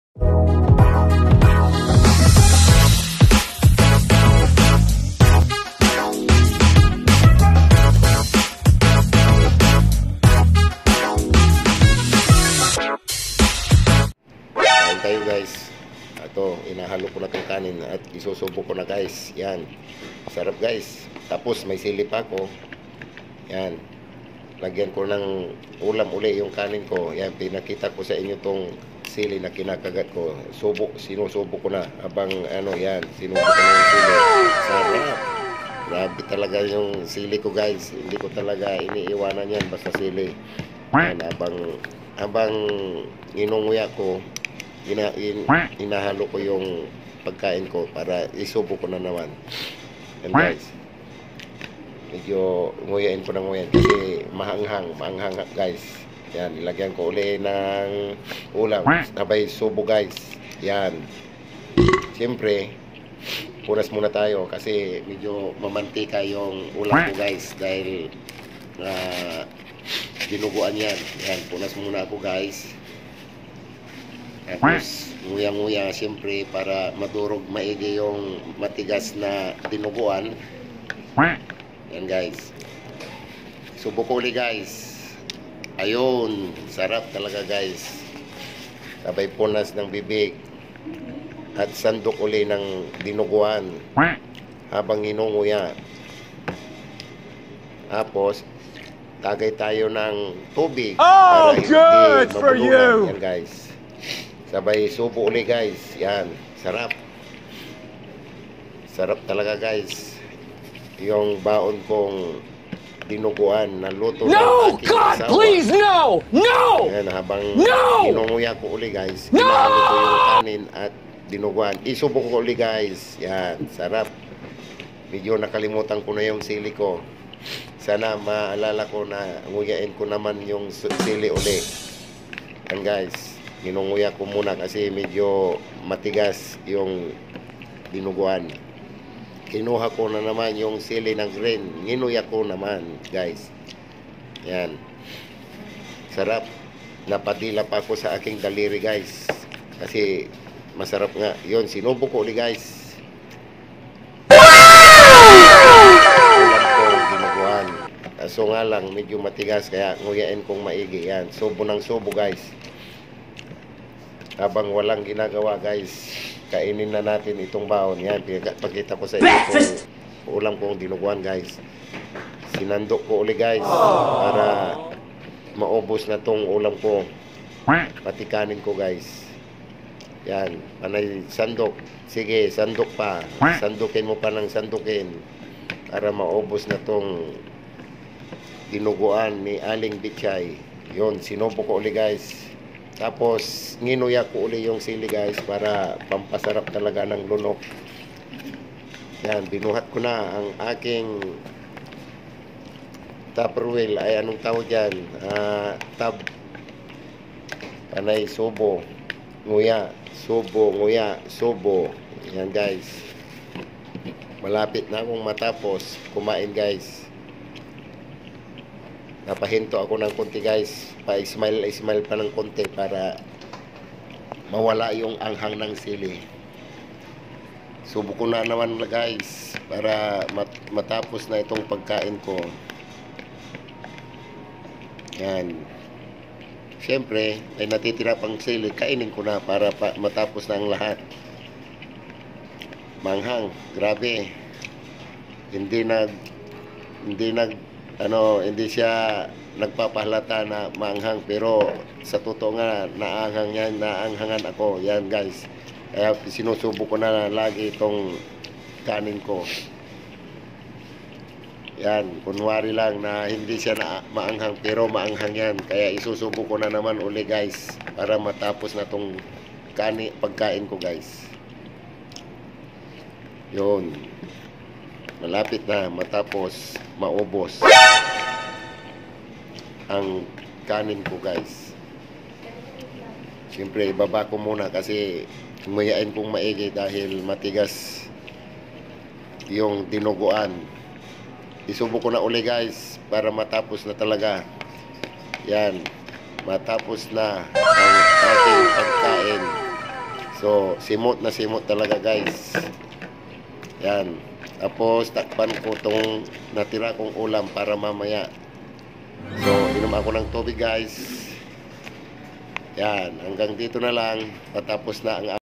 ALMUSAL NA MAY PANULAK NA SILING LABUYO. PANUURIN NIYO GUY'S YONG SILI GINAWA NG MANI.
masarap kumain pag may siling labuyo